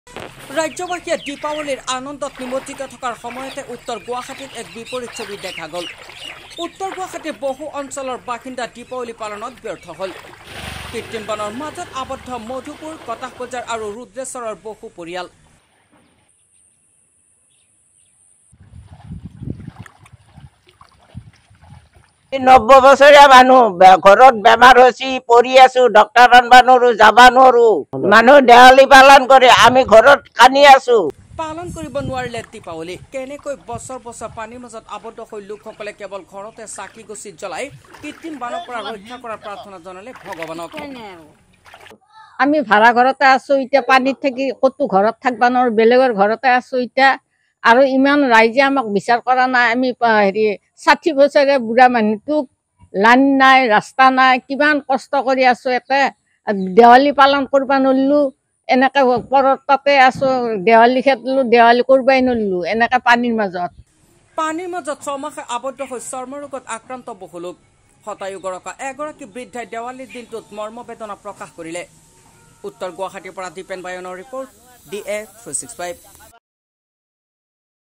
ৰাজ্যবাসীয়ে দ্বীপাৱলীৰ আনন্দত নিমজ্জিত থকাৰ সময়তে উত্তৰ গুৱাহাটীত এক বিপৰীত ছবি দেখা গল ।
today assam 2024
today nalbari 2024
nalbarir khabar
asomar khabar
asomiya news